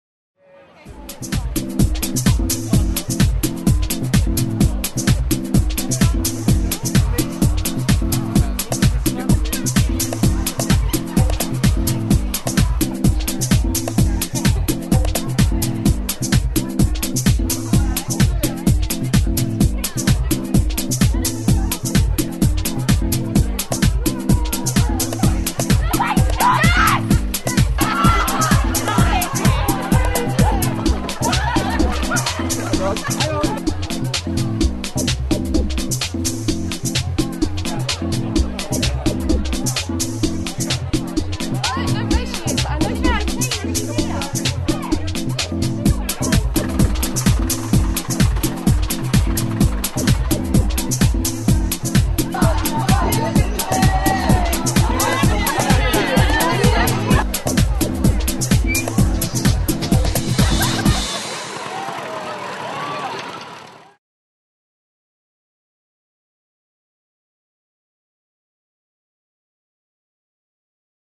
Cantell School prom
Pupils from Cantell Maths and Computing College celebrated their end of school prom at Southampton's De Vere Grand Harbour Hotel.